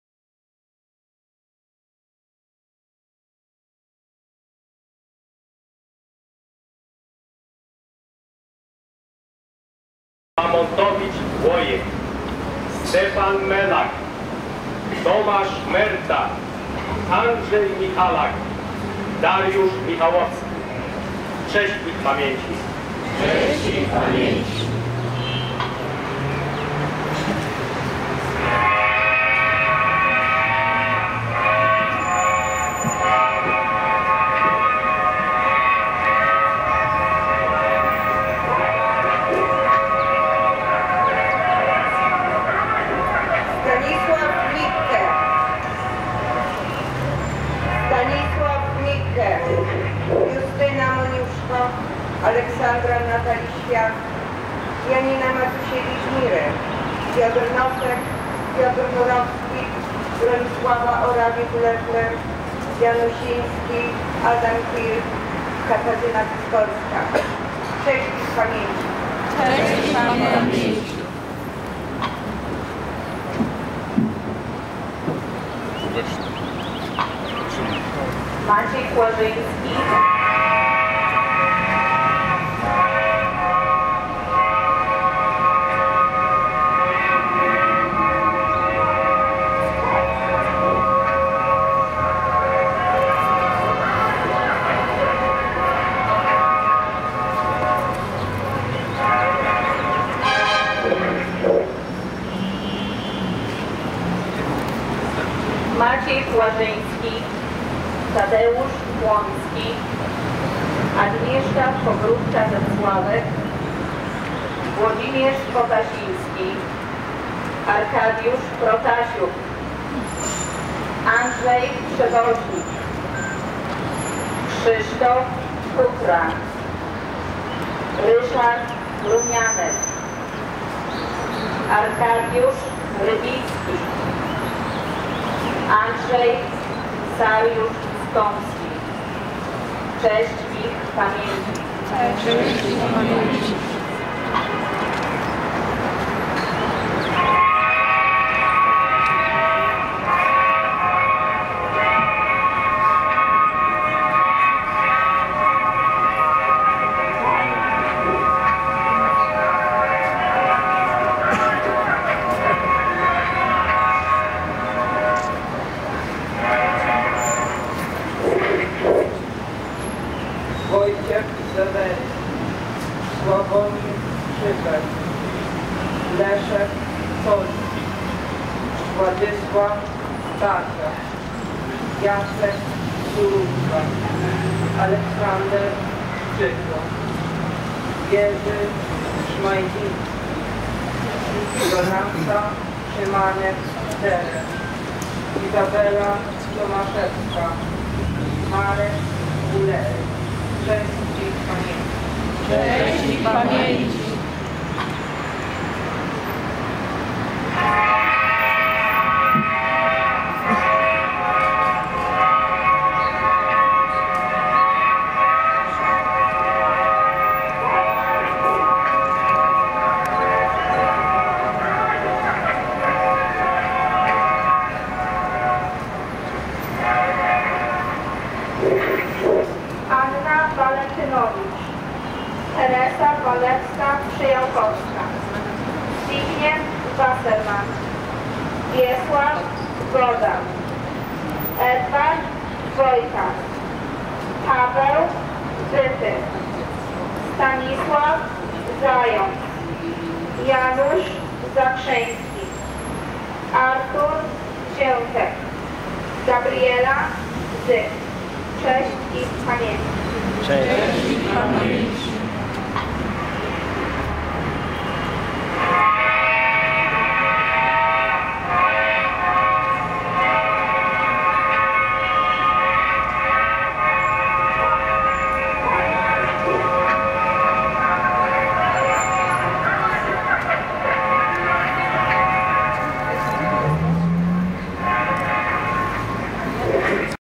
Pierwsza Rocznica Tragicznego Lotu. Smolensk 10 Kwiecien 2010
Uroczyste Odczytywanie Nazwisk Wiec Przed Konsulatem RP w Nowym Jorku 10 Kwiecien 2011 Fragment Filmu 1
Copyright